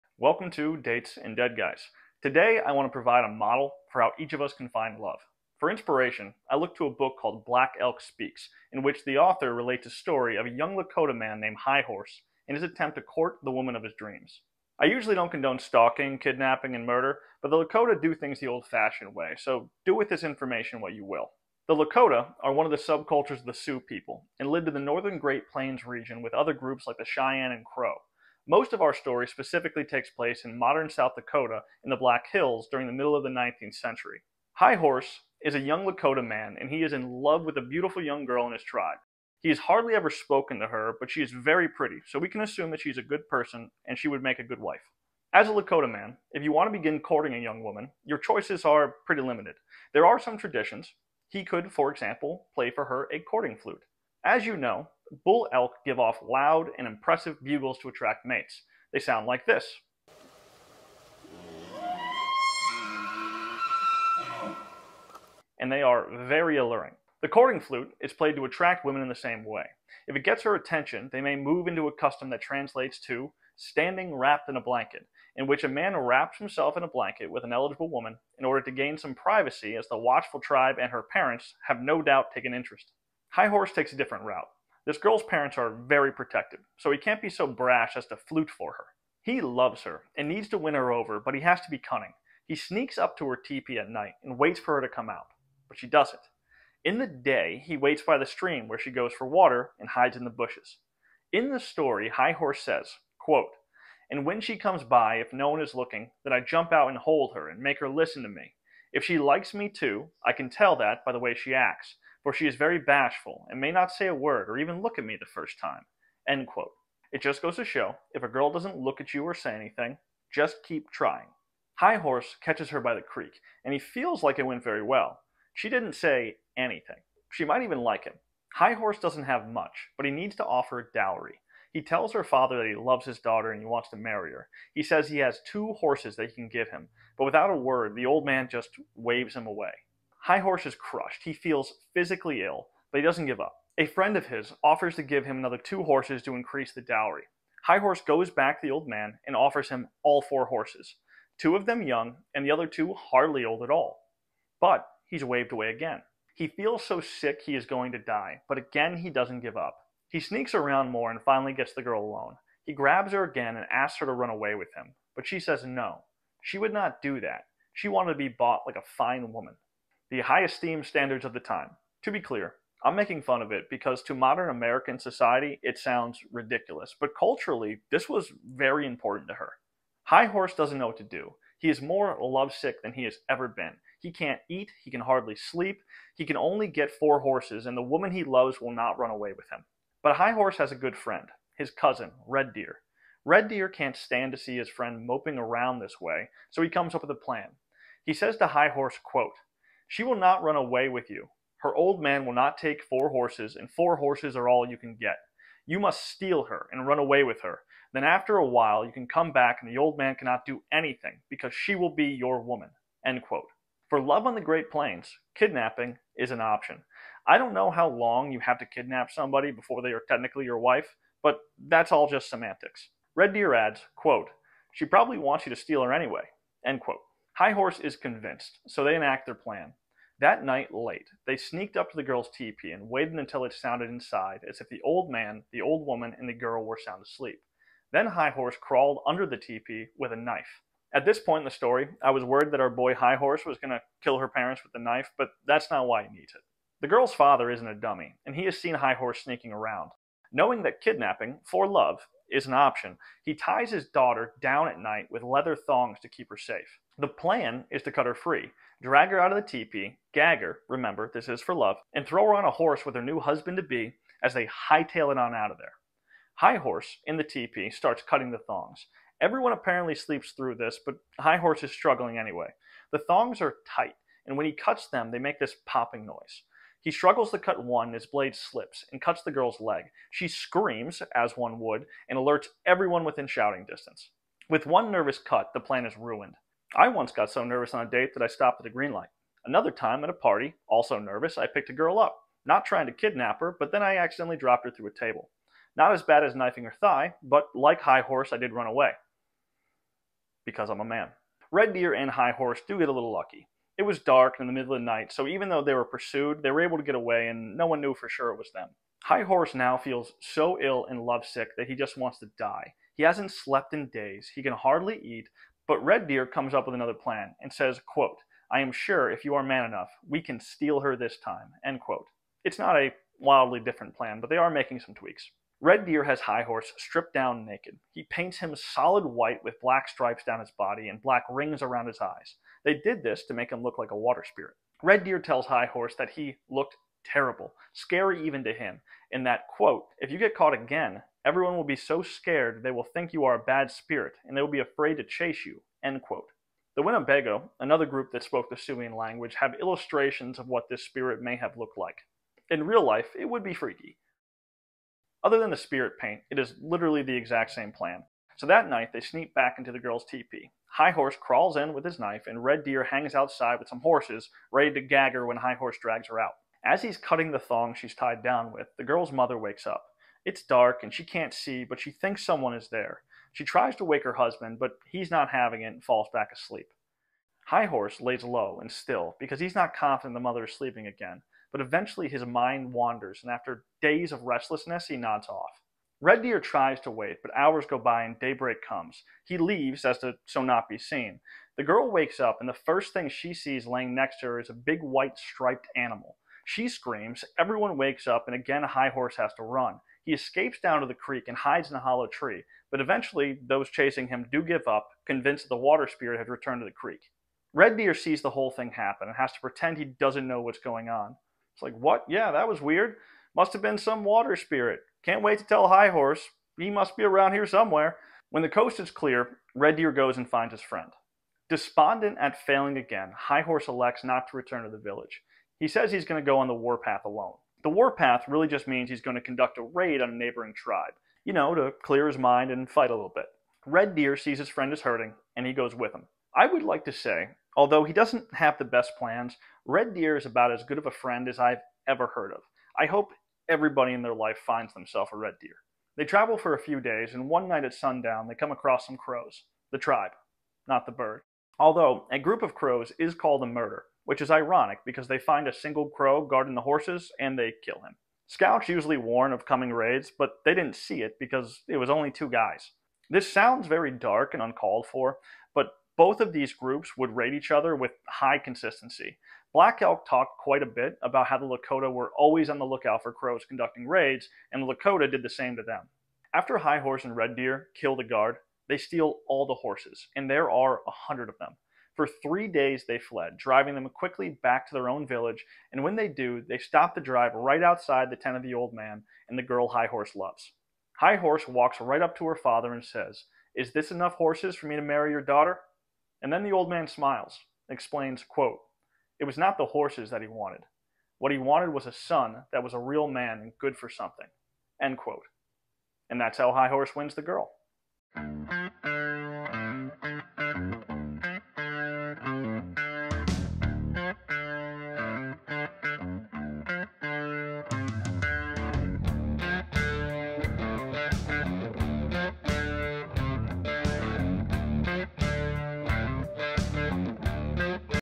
A Lakota Love Story: How to Win the Girl on the Great Plains
This video was inspired by Chapted 6 of Black Elk Speaks in which the author of the book relates a story from Black Elk, a Lakota medicine man, about a young man named High Horse. High Horse is in love and the chapter tells the story of what a Lakota man may have to go through in order to win the woman of his dreams. Its a fun story.

There is some controversy in regard to the book, Black Elk Speaks. It was written in 1931 and the author, John G. Neihardt based the book off of his interviews with Black Elk, a Lakota medicine man. But some of the book raises questions because some of the topics addressed by Neihardt don’t align perfectly with Lakota culture. It’s possible that in the translation of the interviews he made mistakes or that he changed some items to better appeal to his audience. It’s unclear. But this story is an interesting one and a fun window into the life of the Lakota.

I hope you enjoy. If you have any thoughts or comments, please leave them below.

Resources

Black Elk Speaks by John G. Neihardt (Book)

Courting on the Plains: 19th Century Lakota Style by Kathryn M. Duda